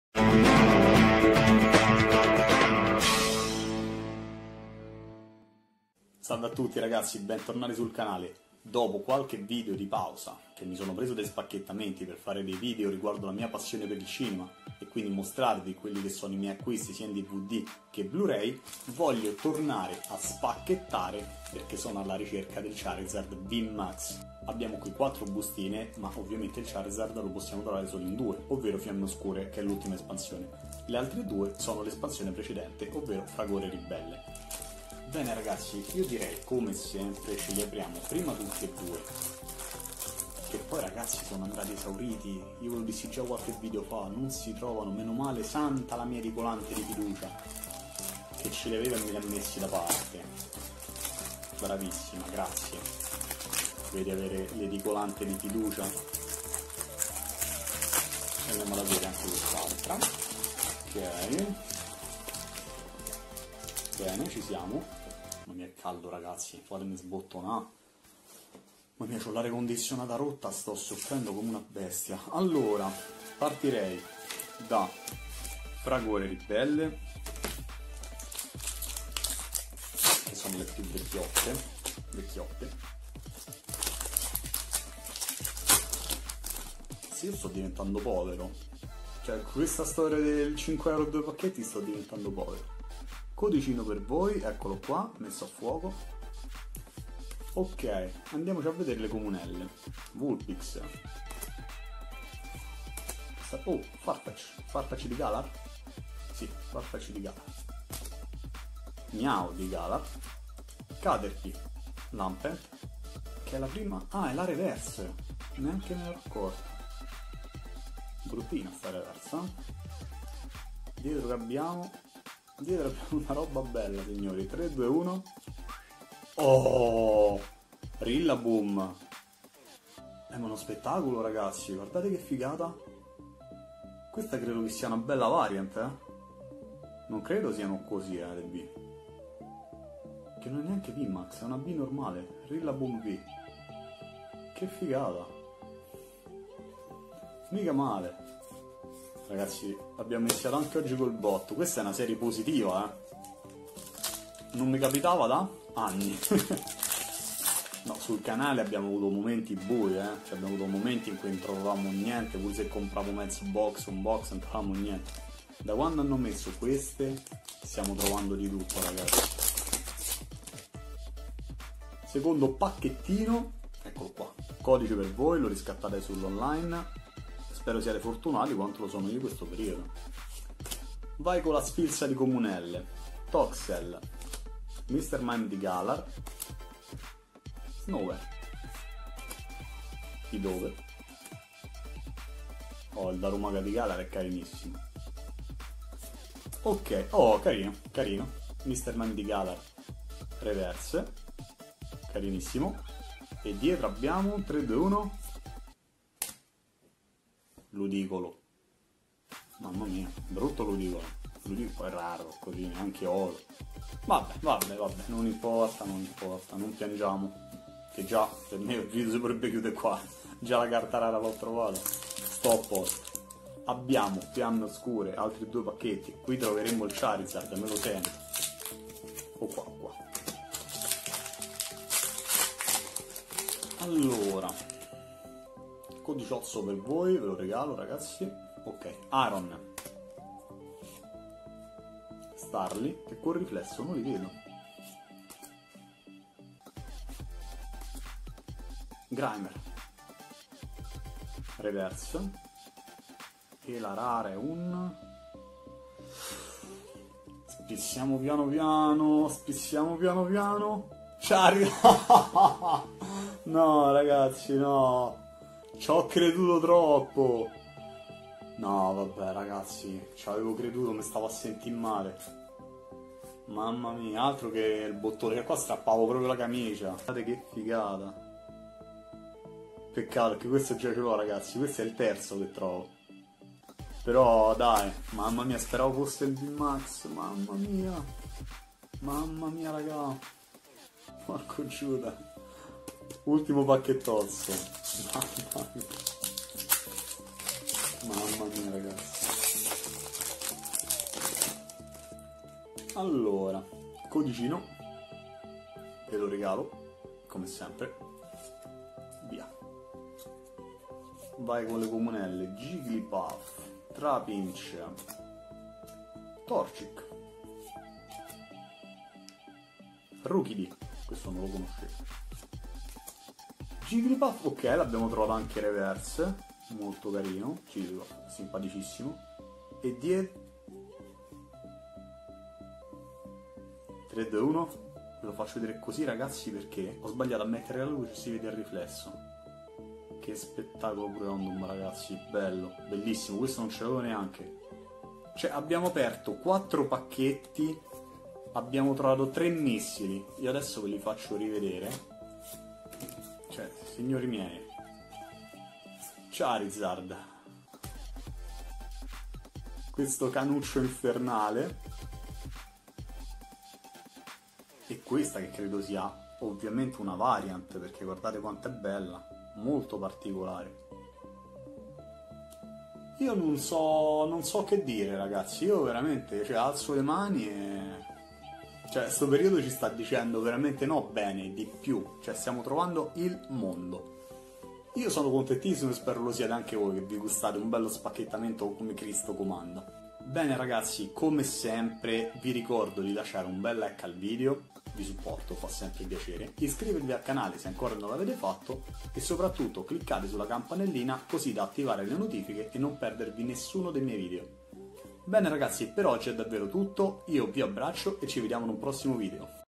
Trovo un "V" in ogni pacchetto!!! 😍 - Pokémon: Spada e Scudo - Fiamme Oscure
Salve a tutti ragazzi e bentornati sul mio canale!!! 😃 In questo nuovo video andremo ad aprire due blister della nuova espansione "Spada e Scudo - Fiamme Oscure" 💣 Che la caccia ai Pokémon più rari abbia inizio 💪

● Lascia un "MI PIACE" se il video ti è piaciuto ✔ 👍🏻
● Clicca slla campanella per restare sempre aggiornato ✔ 🔔